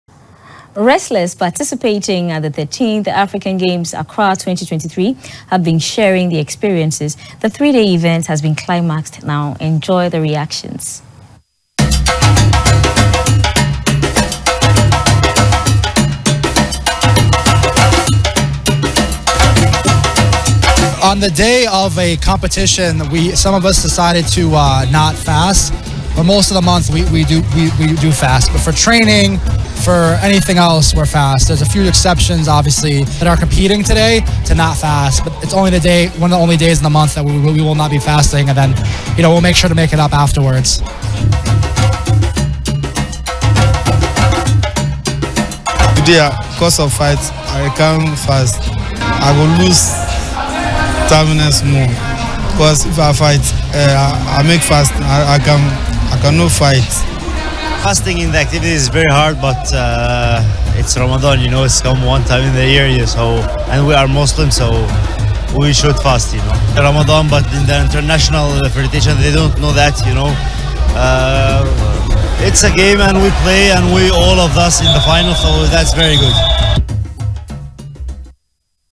13TH AFRICAN GAMES-WRESTLING
Wrestlers participating at the 13th African Games- Accra 2023, have been sharing their experiences. The three-day event has been climaxed. Here are some reactions